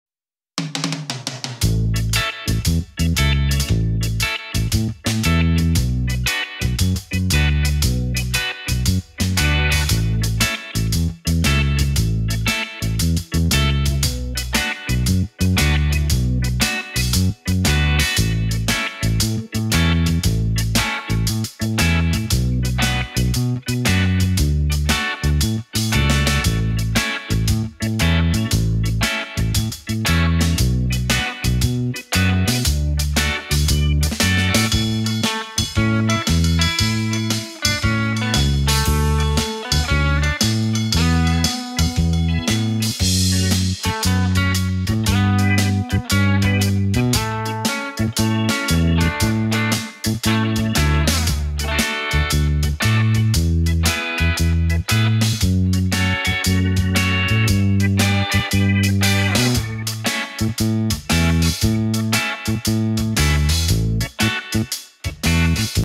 XIAOMI Mi Band 3, XIAOMI Mi Band 4 i ciśnieniomierz!
Porównuję wynik prezentowany przez opaski z pomiarem wykonanym przez ciśnieniomierz.